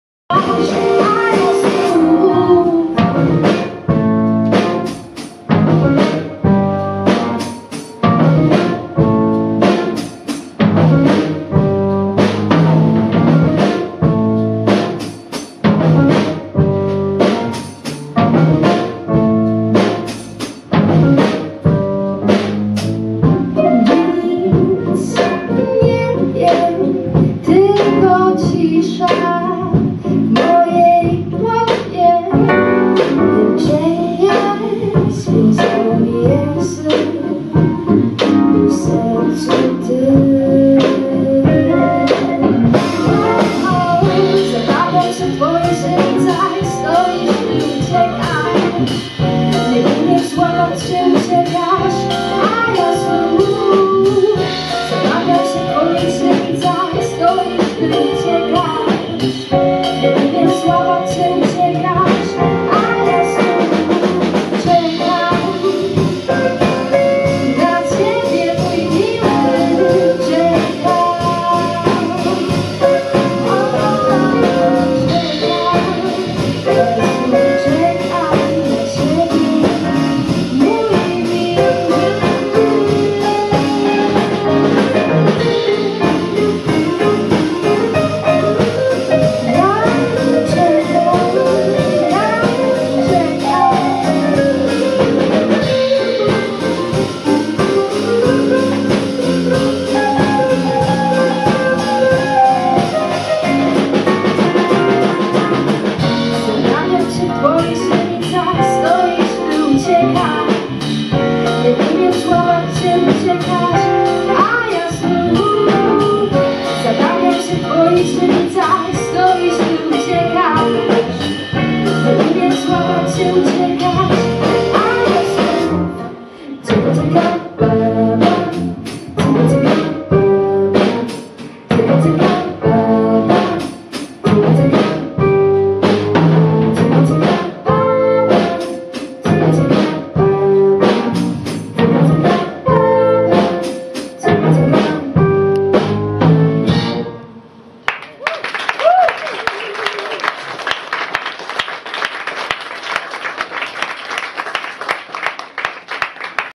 Agata Leśniewska & Loveina Live
Agata Leśniewska (voc), Maja Biesek (g), Jan Jarecki (p), Filip Arasimowicz (gb), Kacper Skolik (dr). Projektowa w Sopocie, 15 czerwca 2019.